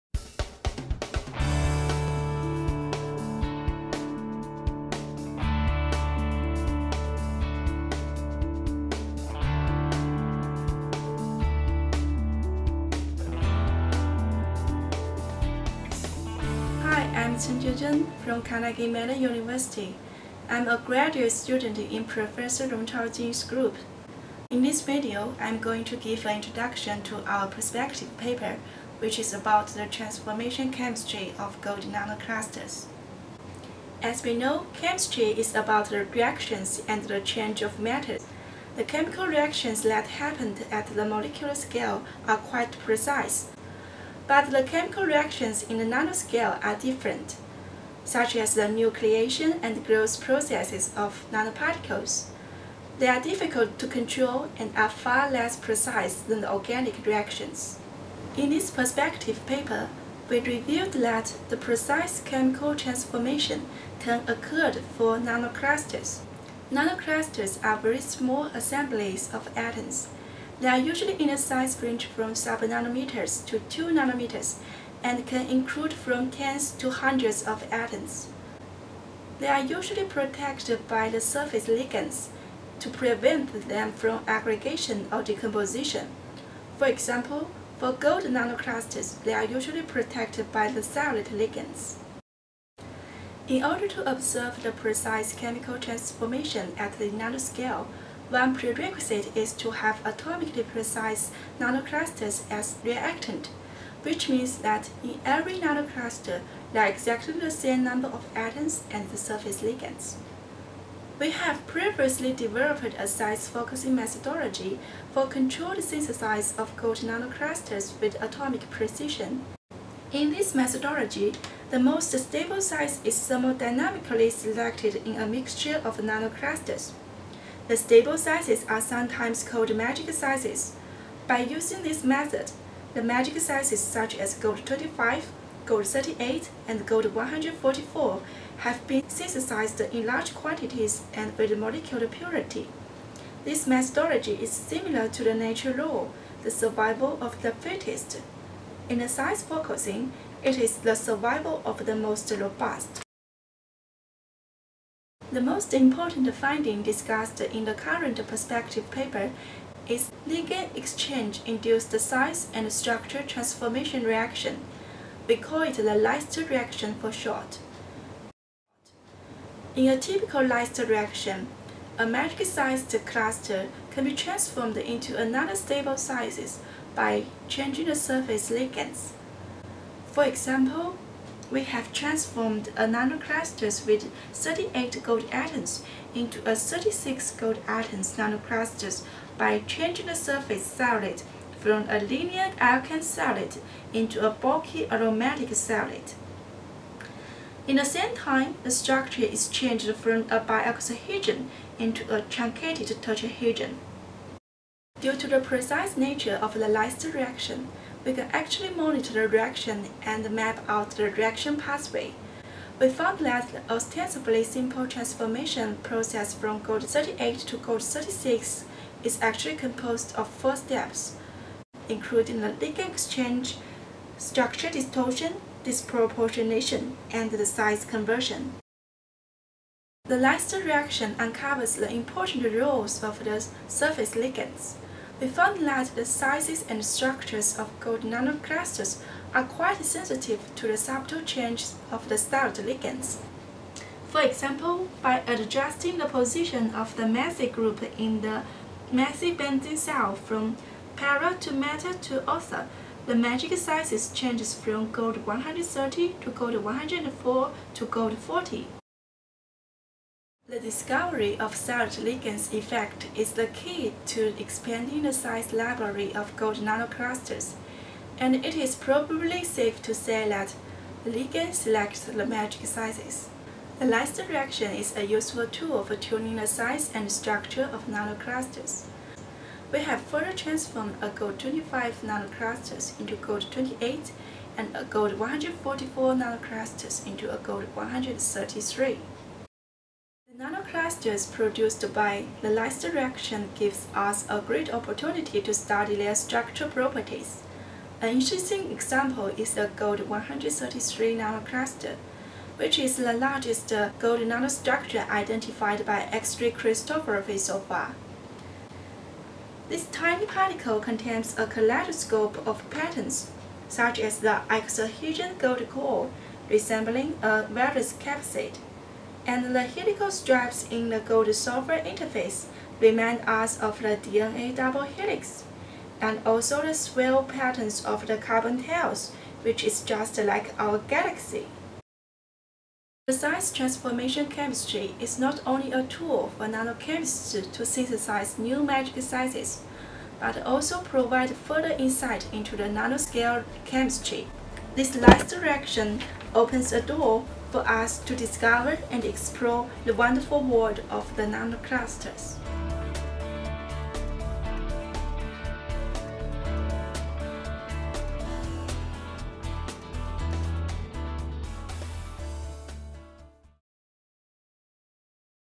Transformation Chemistry of Gold Nanoclusters: From One Stable Size to Another
This Perspective discusses a new methodology for controlling the size and structure of nanoclusters through ligand-exchange-induced transformation of Aun(SR)m nanoclusters.

Featuring: Chenjie Zeng, Yuxiang Chen, Anindita Das, and Rongchao Jin